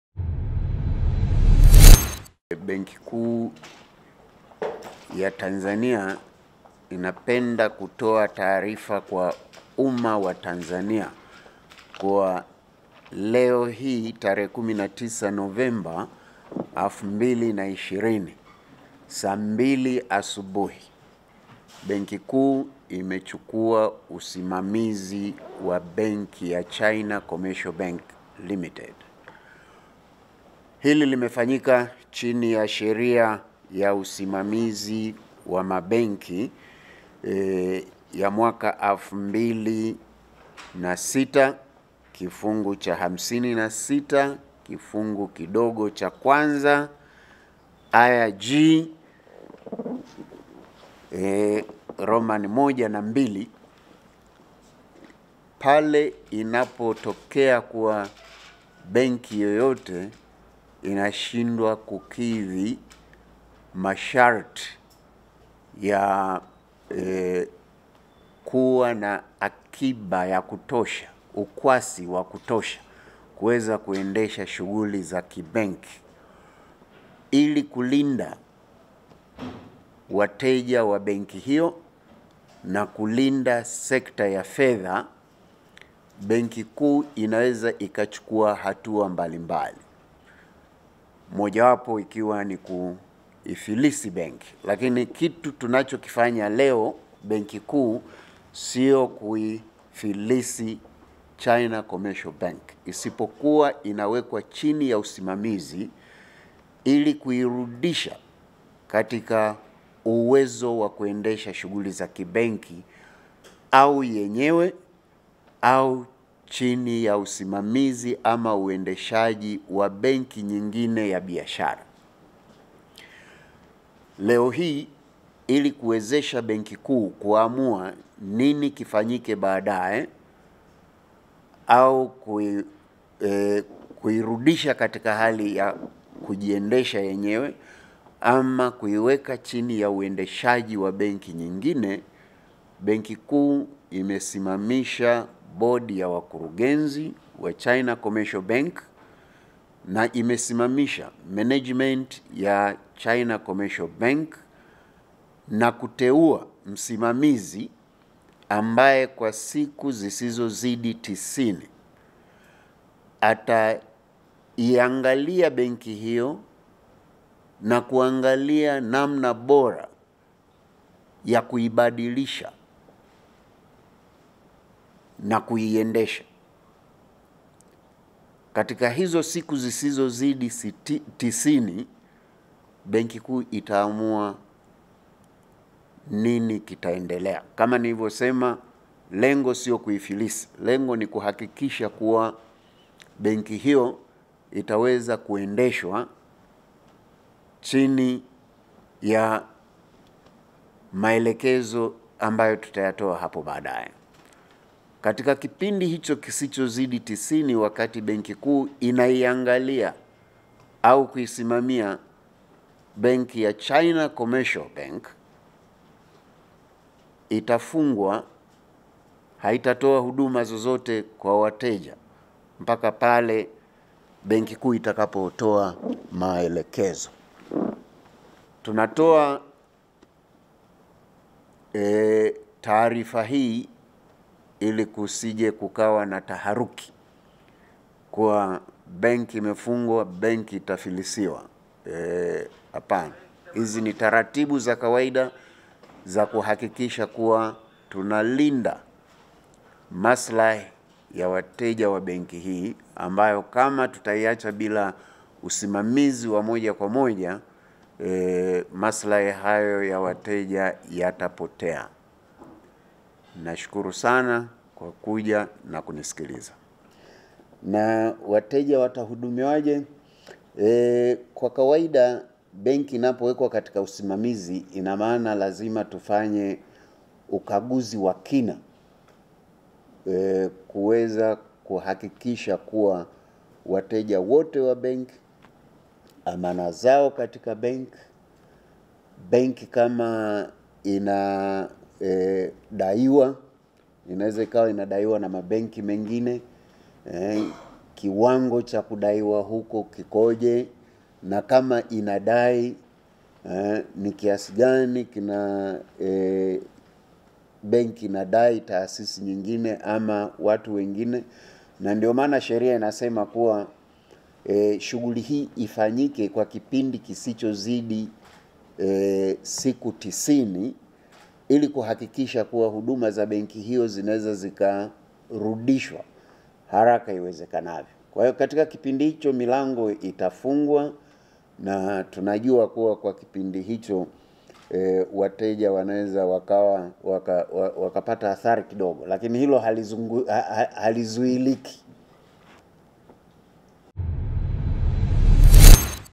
BoT YACHUKUA USIMAMIZI WA CHINA COMMERCIAL BANK LIMITED
Benki kuu ya Tanzania imetangaza kuchukua usimamizi wa china commercial bank limited kuanzia leo November 19

Uamuzi huo umechukuliwa baada ya kubaini kuwa china commercial bank limited kuwa na mapungufu makubwa ya mtaji kinyume na matakwa ya sheria ya mabenki na taasisi za fedha ya mwaka 2006 na kanuni zake.